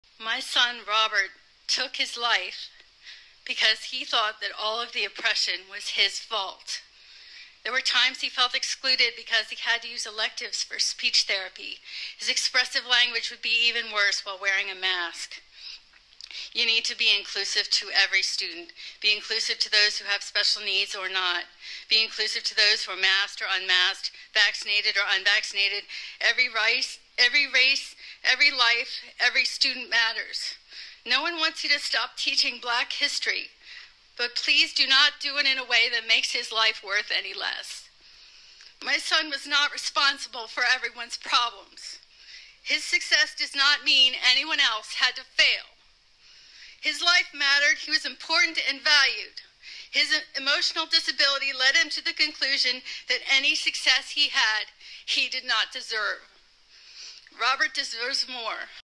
THIS PARENT’S SPEECH AT FAIRFAX COUNTY SCHOOL BOARD IS HEARTBREAKING
Shorts